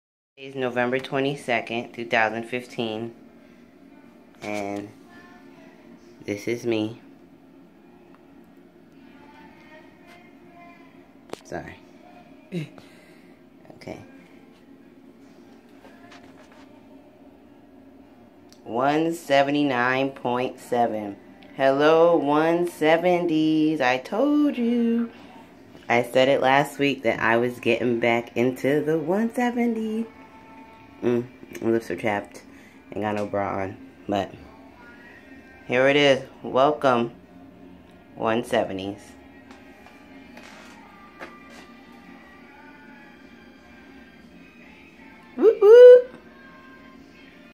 Week 2 weigh in FBF 34 11/22/15
Face the scale Sunday! Did I lose, gain or stay the same?